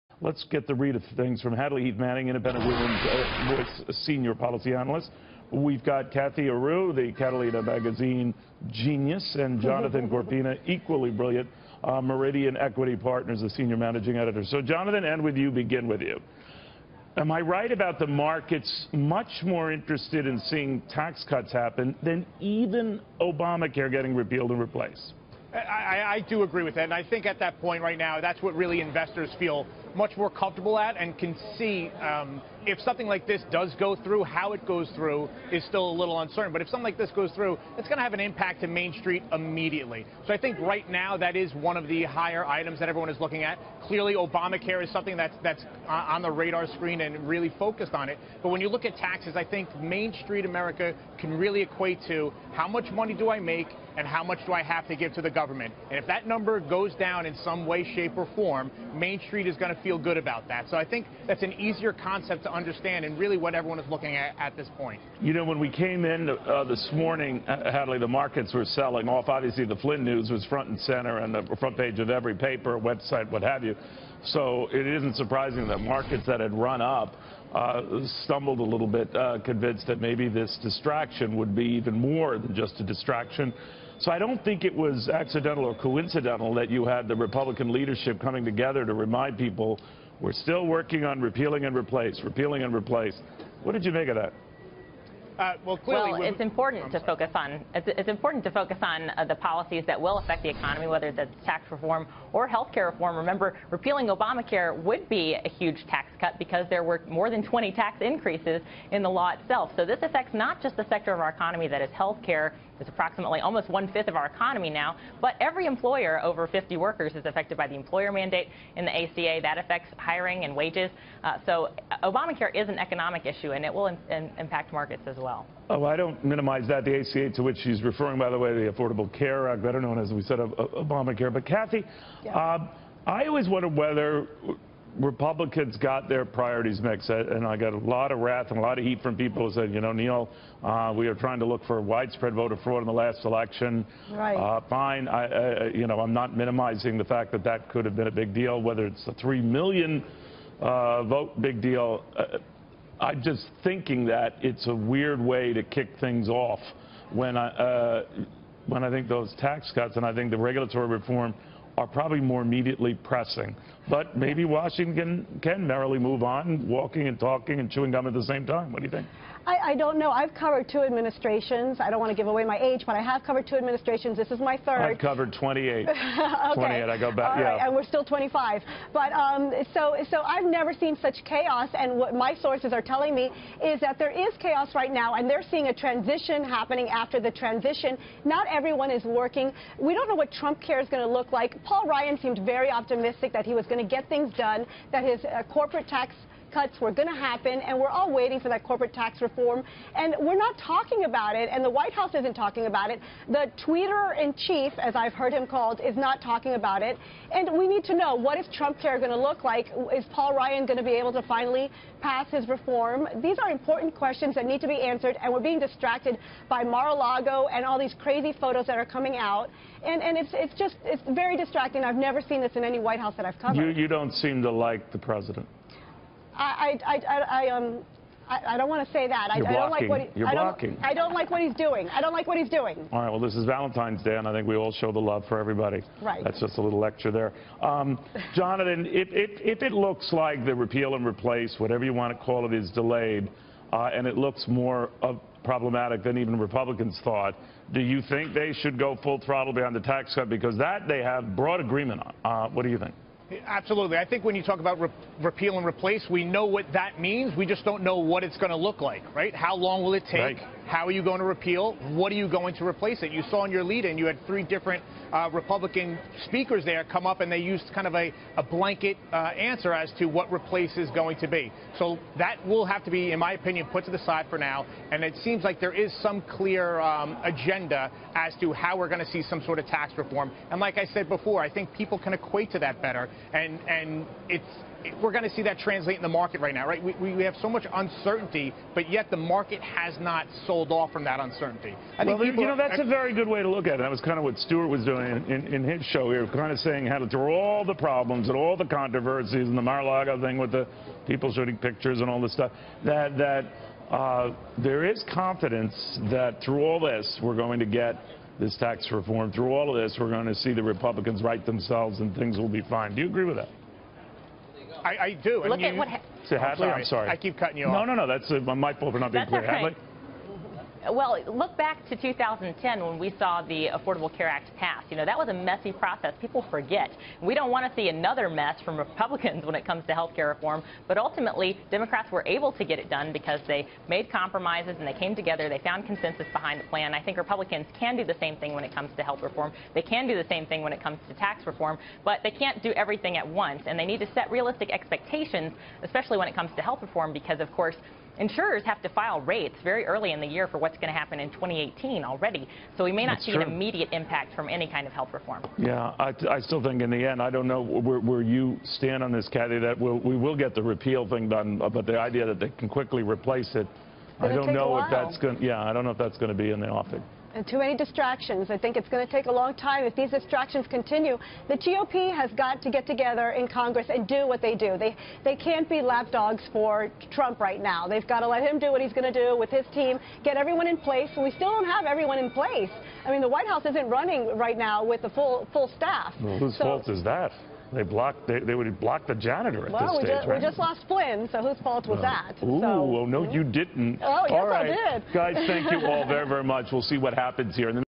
The impact of Obamacare vs. tax reform
Independent Women’s Voice Senior Policy Analyst Hadley Heath Manning, Catalina Magazine Publisher Cathy Areu and Meridian Equity Partners’ Jonathan Corpina discuss why stocks are in record territory amid talks of Obamacare repeal and tax cuts.